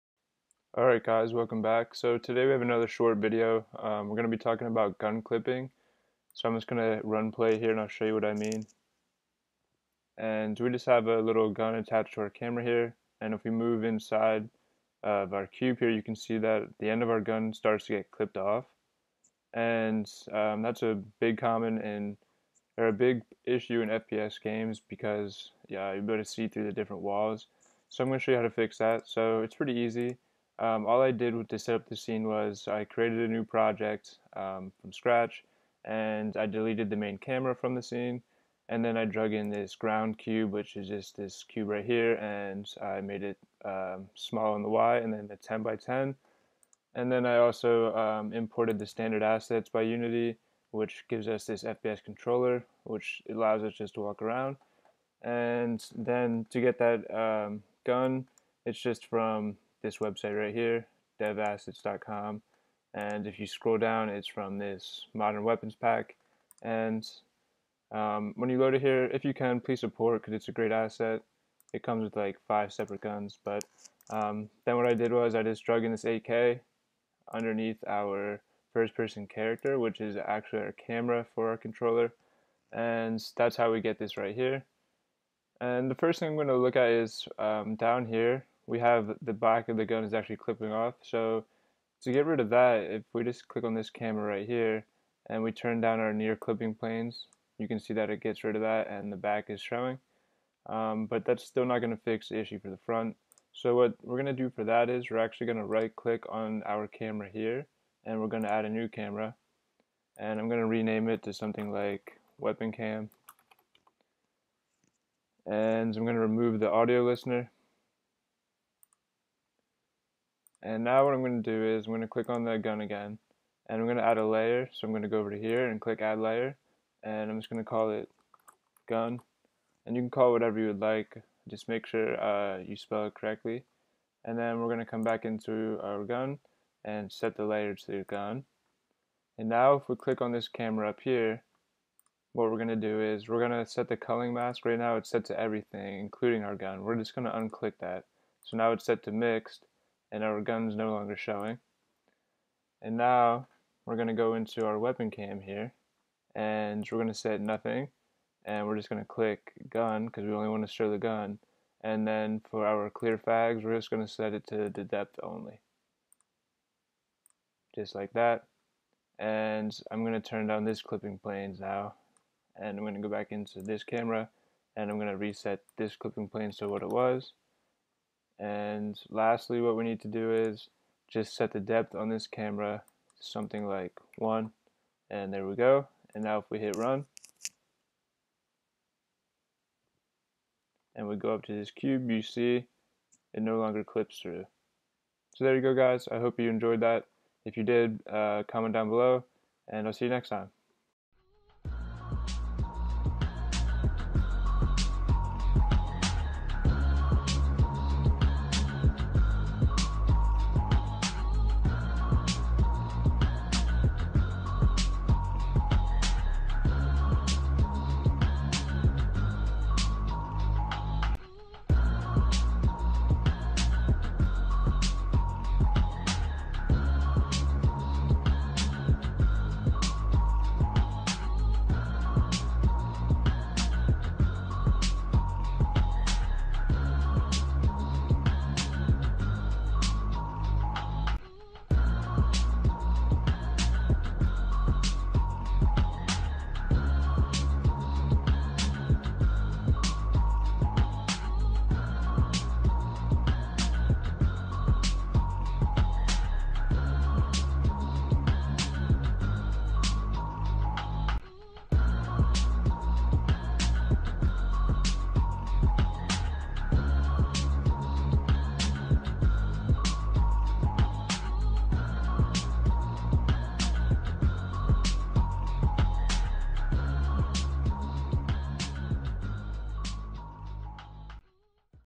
Camera Clipping in Unity3D
In this video we look at how we can have two seperate cameras for our FPS controller. This will prevent camera clipping of the different items that you may be looking at. It is fairly straight forward and mainly involves playing around with the different camera layers!      Links to Dev Assets Pack